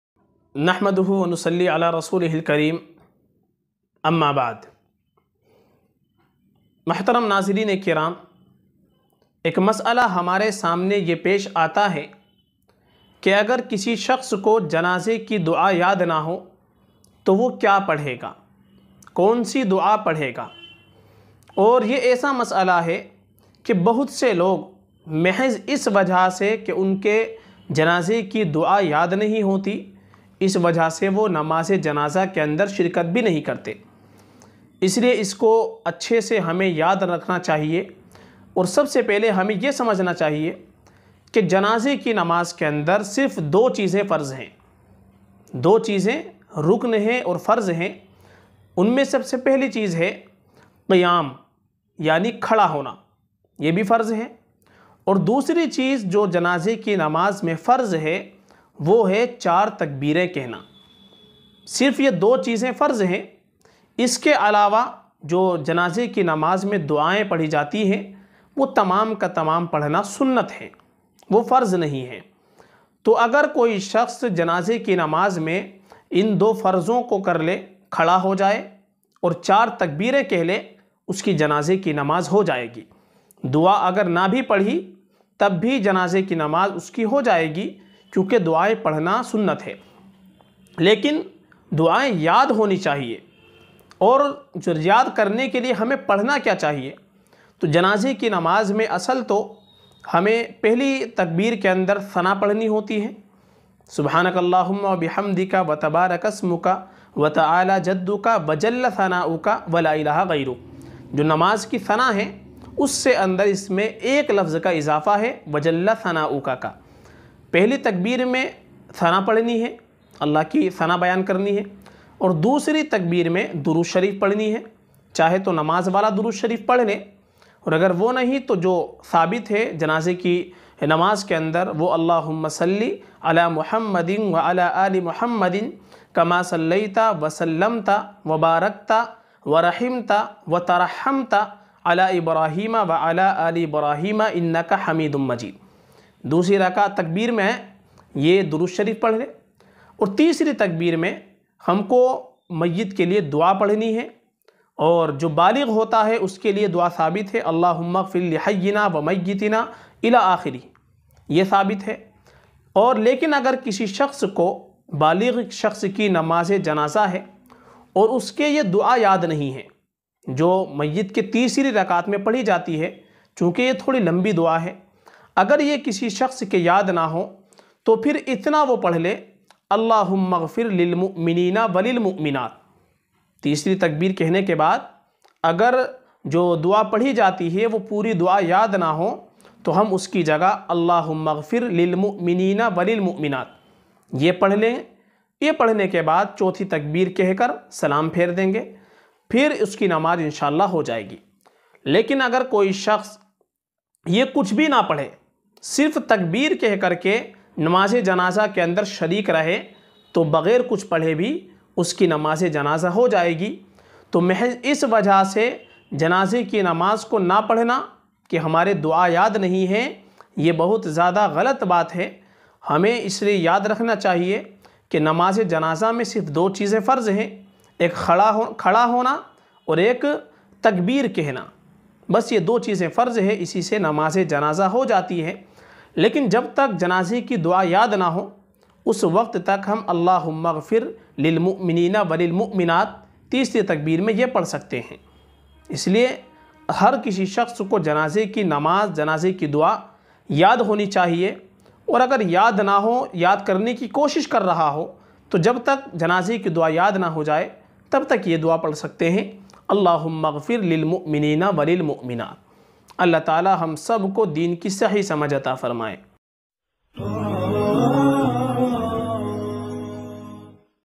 Janaze ki dua yaad na to kya padhe , (very important clip) , by Mufti Sadaqat Husain qasmi
Agar kisi ke janaze ki dua yaad na ho to uski jagah kya padhen, aur namaze janaza me konsi duaein padhi jati hai? In sawalon ke jawab janne ke liye puri video dekhen, by Mufti Sadaqat Husain qasmi

اگر کسی شخص کو جنازہ کی دعاء یاد نہ ہو تو وہ کیا پڑھے گا؟ مفتی صداقت حسین قاسمی